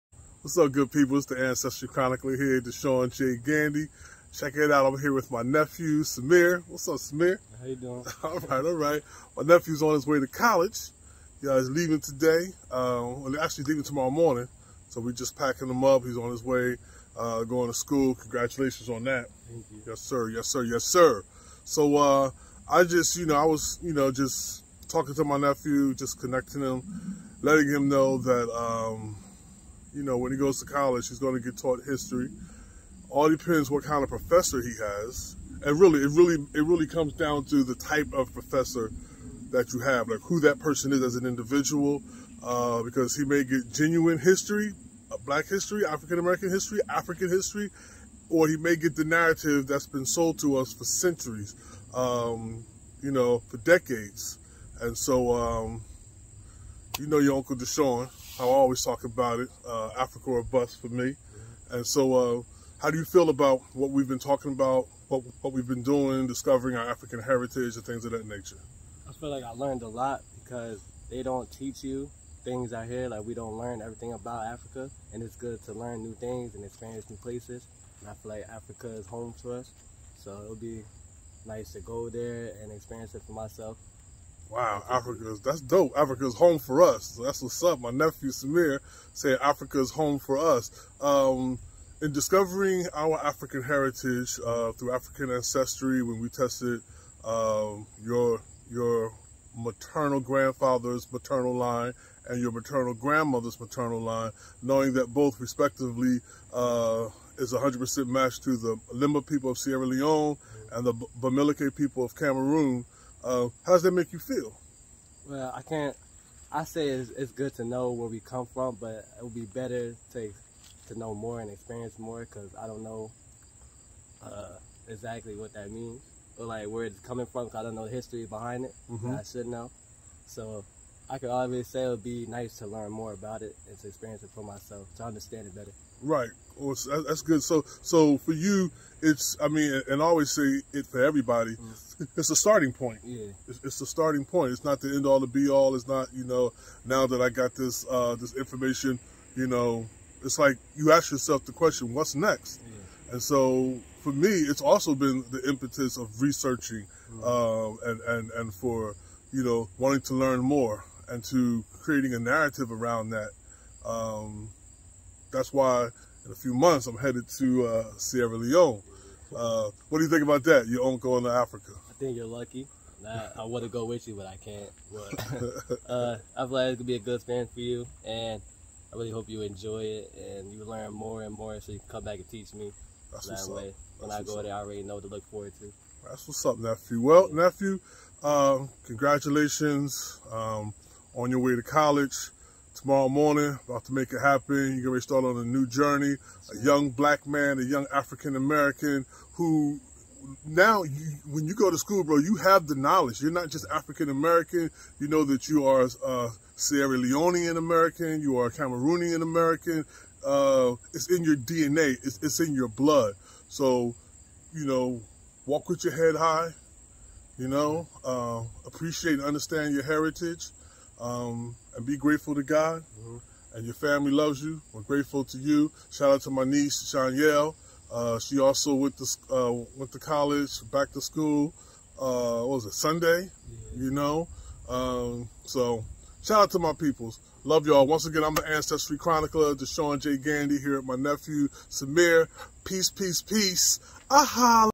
Headed to College with the Power of Knowing his African Roots!
It's that time of year again! Shout-Out to my Nieces and Nephew returning back to college for another year and going to college for the first time! Enjoy! You have what you need to succeed in your DNA, because of your African Ancestral Heritage! Keep God First!